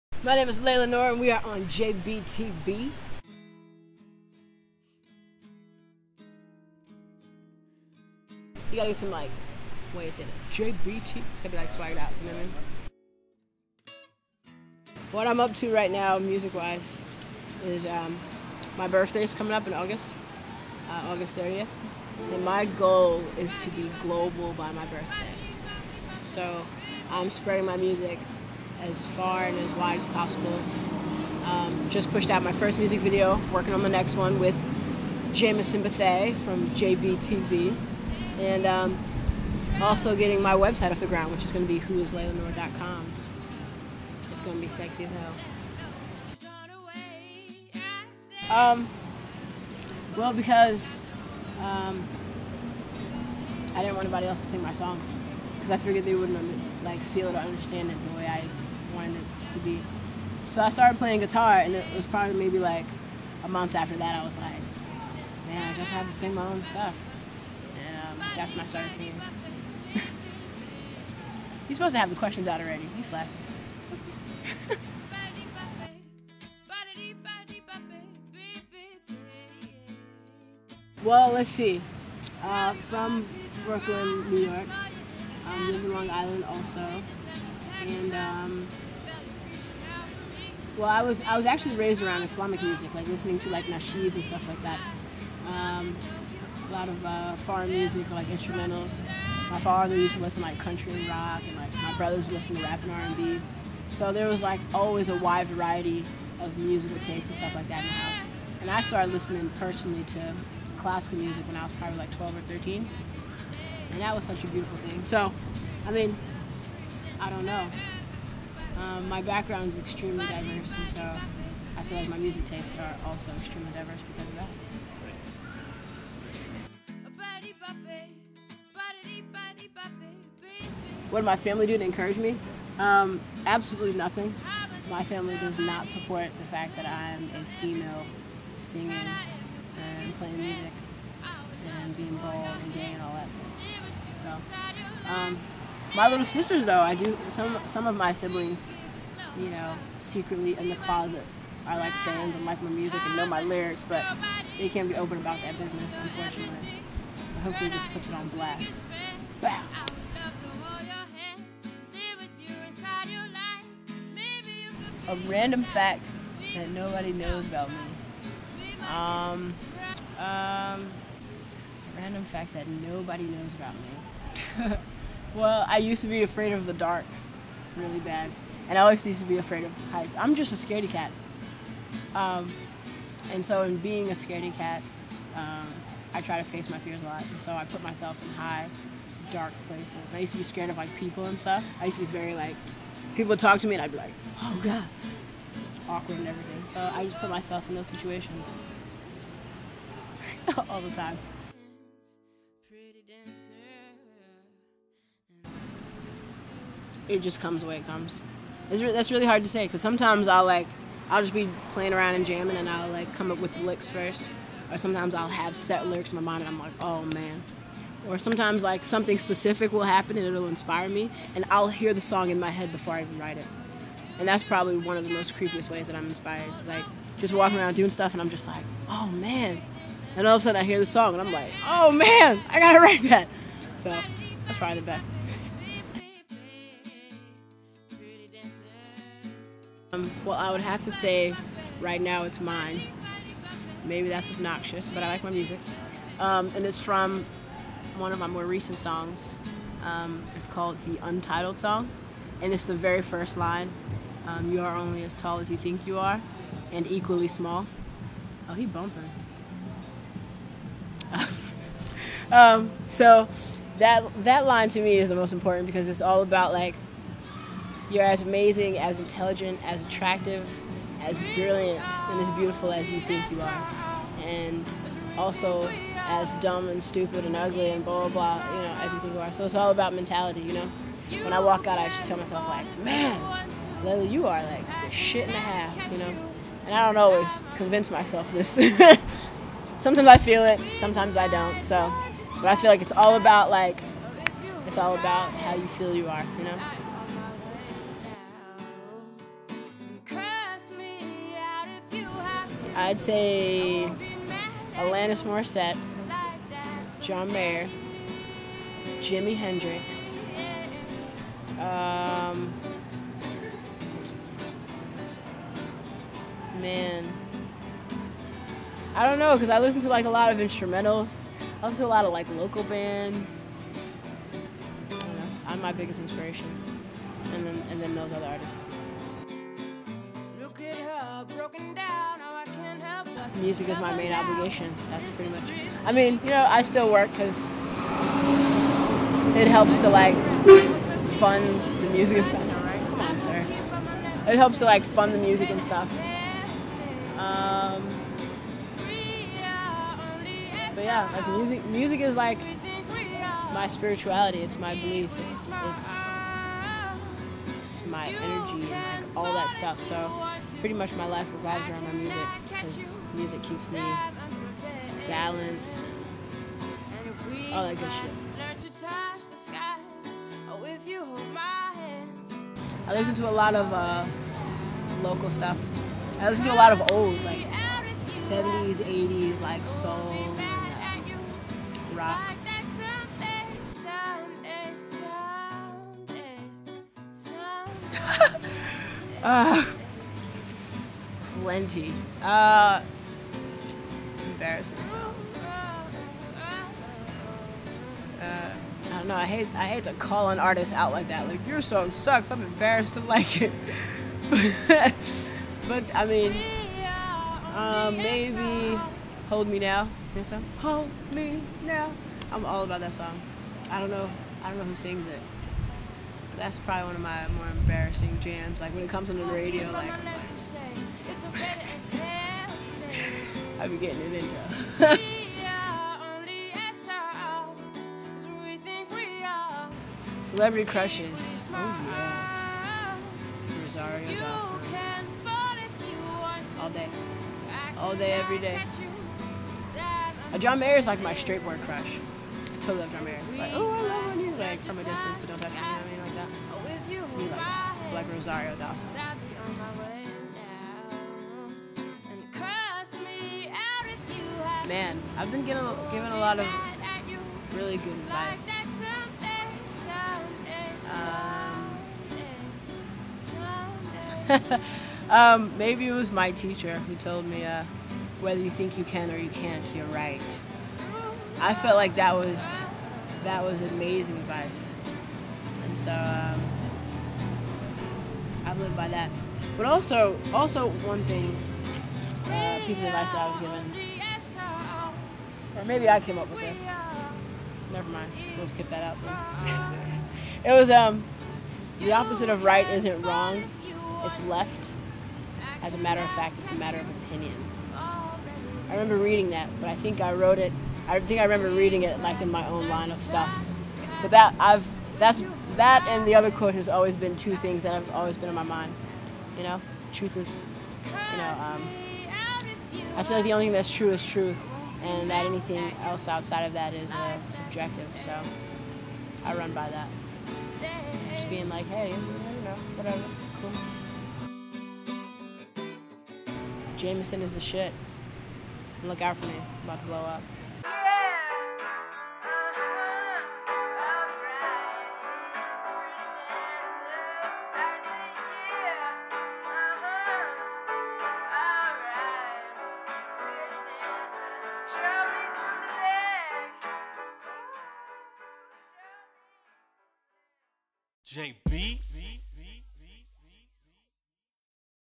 15 QUESTIONS: an Interview with Laila Nur
to view the two questions not featured in the video!

The first of the "15 Questions" interview series!
Produced by Jamison Bethea for JamisonBetheaTV

Bio:
New York native Laila Nur is a self-taught singer, songwriter and guitarist. Currently residing in Greensboro, North Carolina, this talented artist keeps her heart in the youth movement but doesn't categorize her music as being limited to any era. Playing hints of folk, funk, rock, pop, and blues, she simply says, "I'm the music in my head". Laila started playing guitar just recently in September 2009 and is now well known for making up chords and catchy songs about life, love, and politics that gather crowds from every walk of life. Peace :)